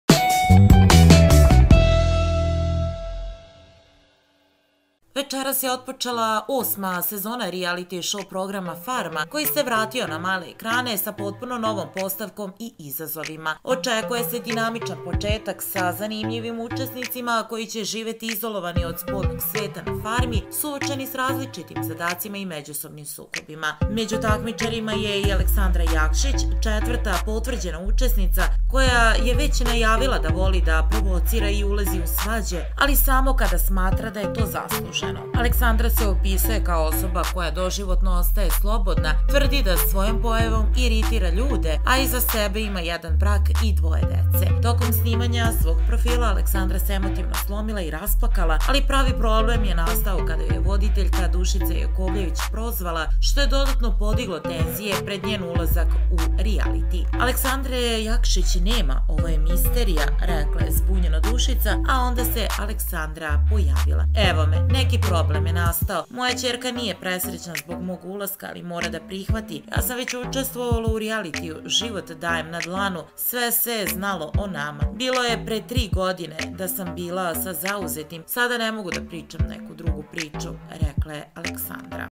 NEVIĐENO - BEKSTVO UOČI ULASKA u RIJALITI
Listen to my opinion about Serbian showbiz, Zadruga & Elita latest news - My reaction
Poslušajte nove vesti, zanimljivosti, životne priče, sudbine, Zadrugu, Elitu itd